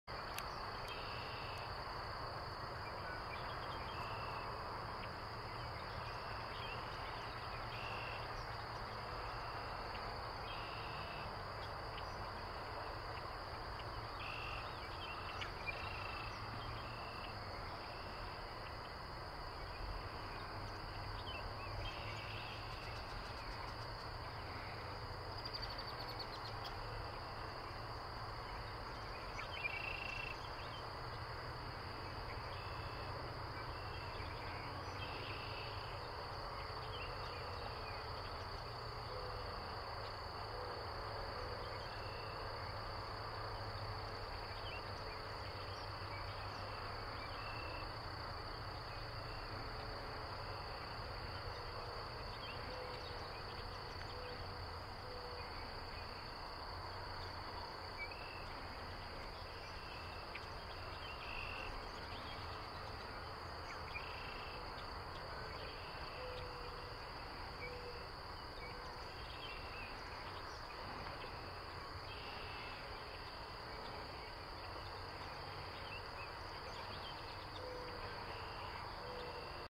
Cottontail rabbits fighting, Baker Wetlands, Lawrence, Kansas
Cottontail rabbits having a squabble and dust-bathing, while an Indigo Bunting, a Brown Thrasher, and other birds go about their business. Baker Wetlands, Lawrence, Kansas. May 28, 2017.